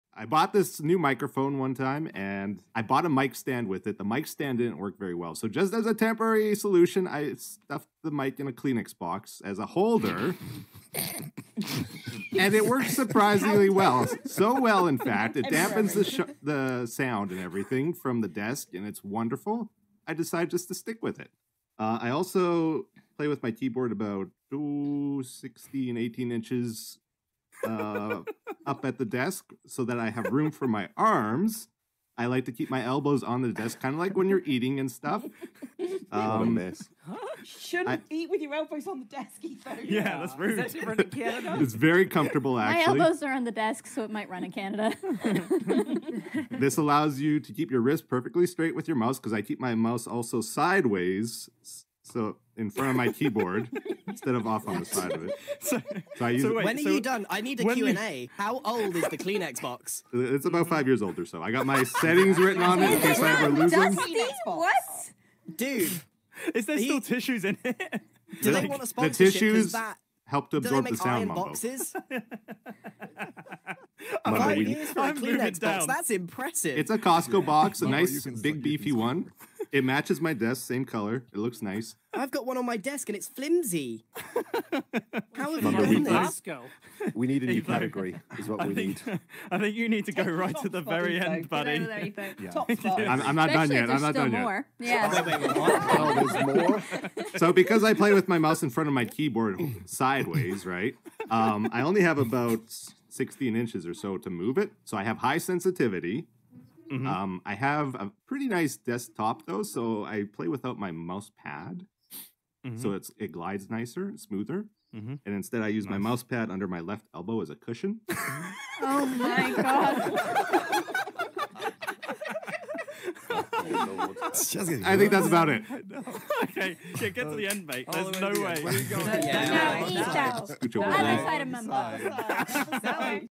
PHOTO OF ETHOS DESK SETUP - Revealed
Etho explains his desktop setup (photo included) to the HermitCraft server.